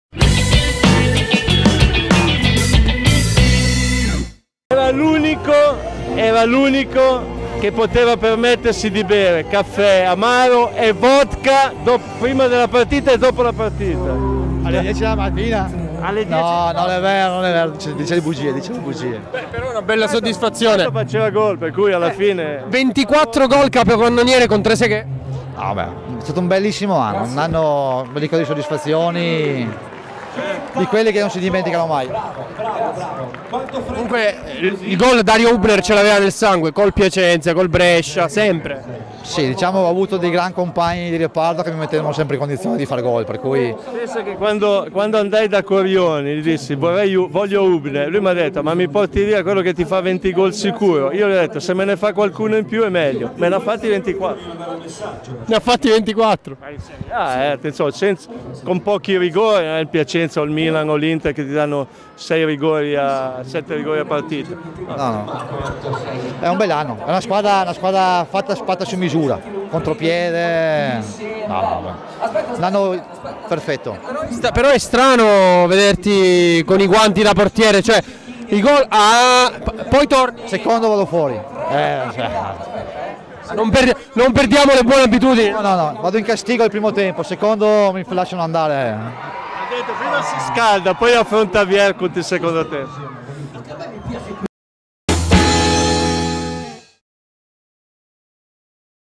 Collovati a Hubner: "Io so qual era il tuo segreto per segnare tanti gol, lo sveliamo?"
12/10/2016 - Divertente siparietto tra Collovati e Hubner ai microfoni di Calcissimo.com!